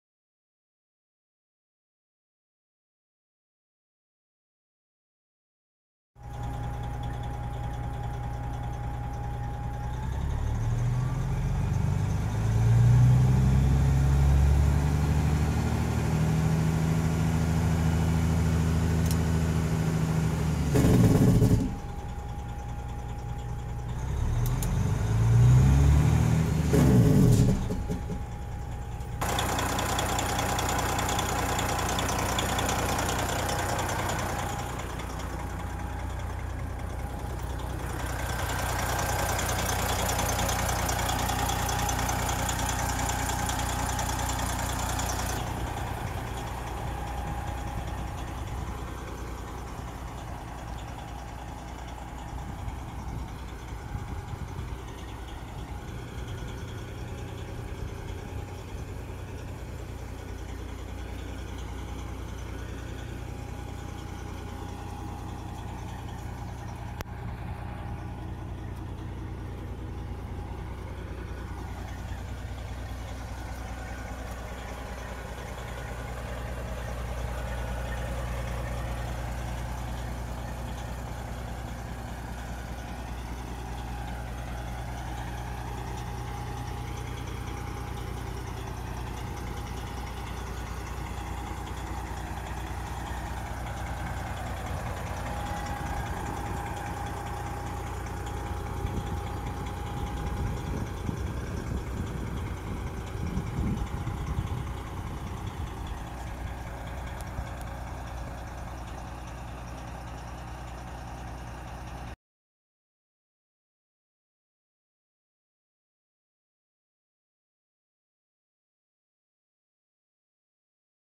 Up for sale 1999 International Eagle 9900i ELD EXEMPT TRACTOR TRAILER SEMI TRUCK
Tool Only Dewalt DCF899
Complete Set
Batteries Pair

Up for sale our company truck International Eagle 9900. Comment for any questions